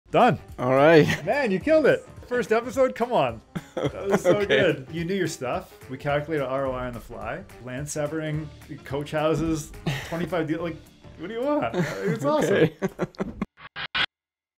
Coach Houses. The Icing on the Cake w/ Vlad Maevskiy
Vlad Maevskiy started investing in Real Estate in 2005 and got into it by accident. He owned 1 investment property passively in North York for 10 years.

The property more than doubled in price, which allowed Vlad to become an active and strategic investor and start building a rental portfolio in hot markets in Ontario. He became a REIN member, and spend hundreds of hours educating himself on various strategies and deep knowledge surrounding the Real Estate business. Coach Houses are the icing on the cake.